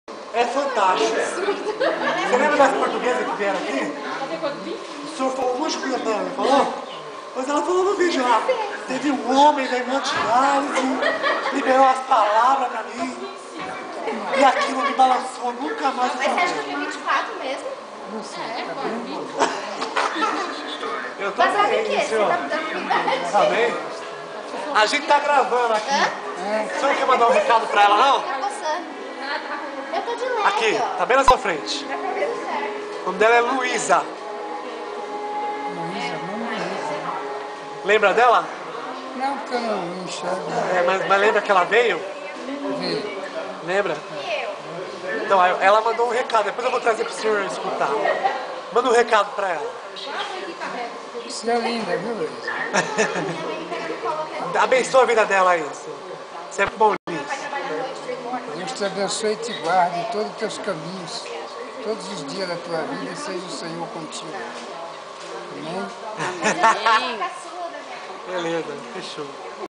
Sr. José Maria abençoando a Luisa em Portugal
Paciente da Hemodialise do Hospital Geral de Carapicuiba/SP - Brasil abençoando a Luisa Ferreira nossa amiga e parceira do Liga da Vida.

Foi muito legal esta ultima visita do ano, foi especial mesmo!
E filmar o Seu José Maria foi emocionante.